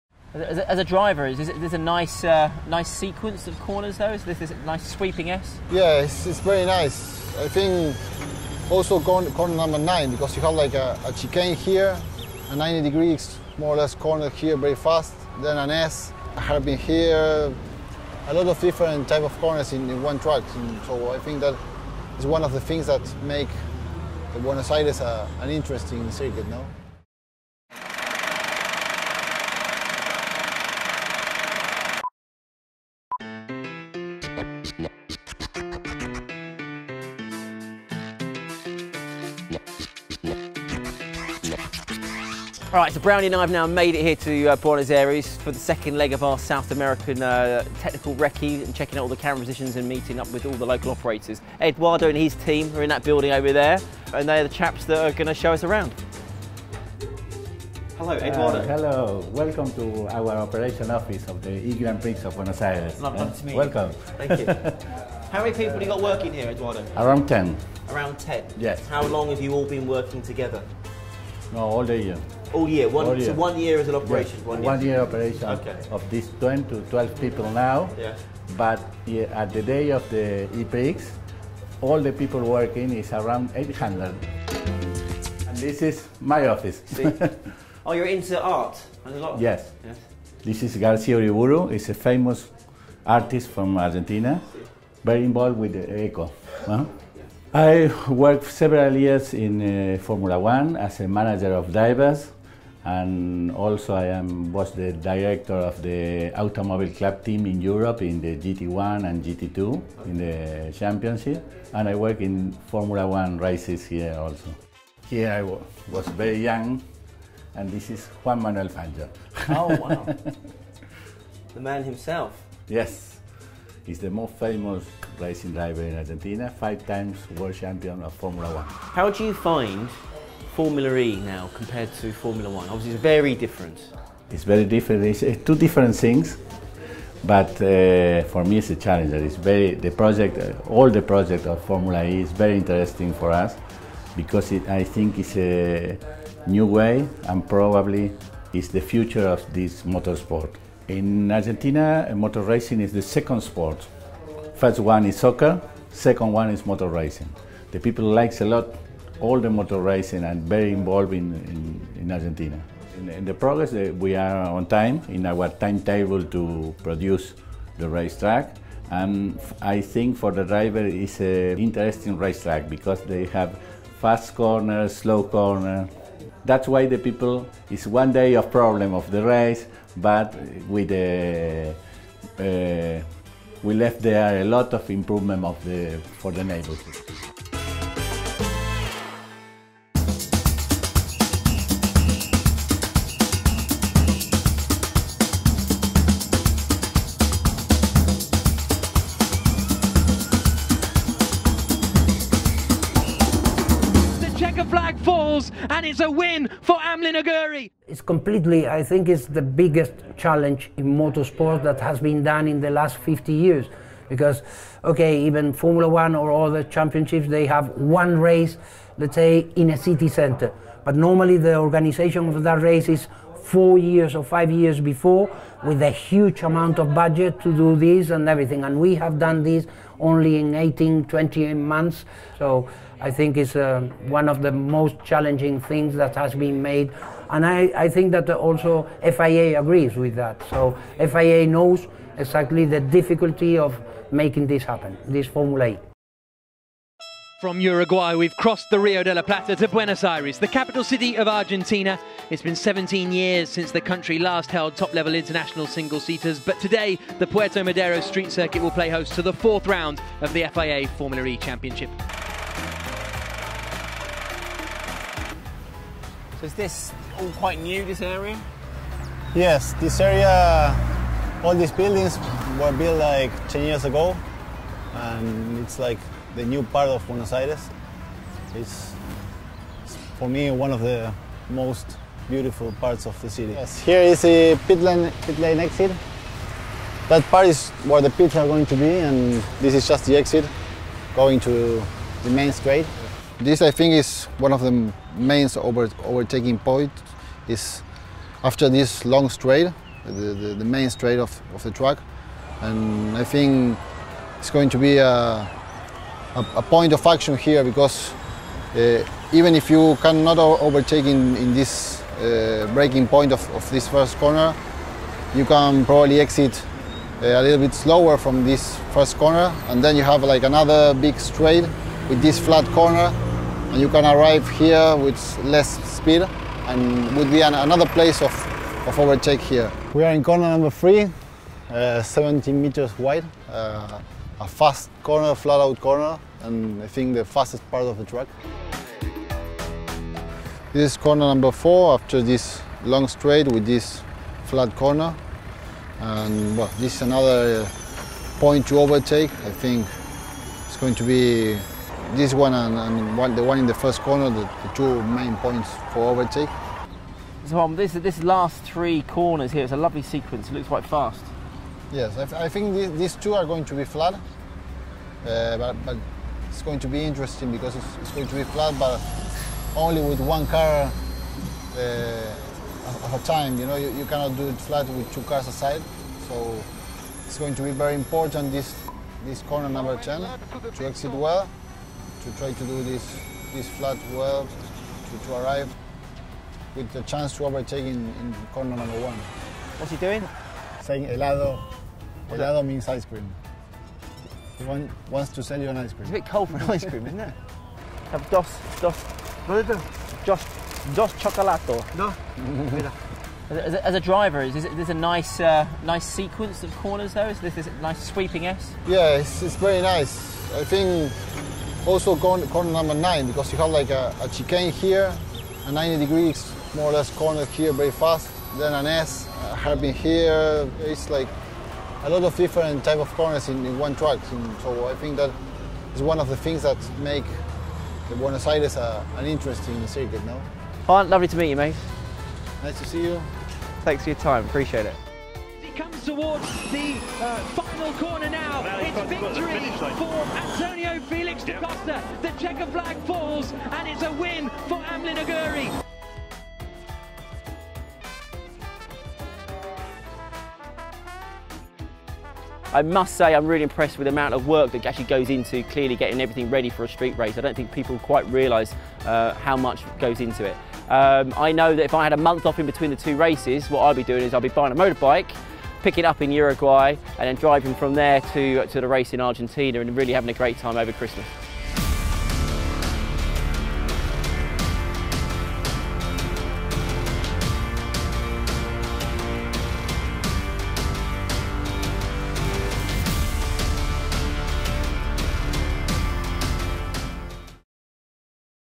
Inside Formula E: Creating A Street Race In Buenos Aires - Formula E
Formula E series director Westbury Gillett has filmed motorsports events the world over, in every championship imaginable. The complexities of staging any televised motorsport event are huge, but on a temporary street circuit in the heart of a bustling city like Buenos Aires, those logistics are simply staggering. Find out what goes into putting on the show you'll see beamed live from the streets of sunny Argentina.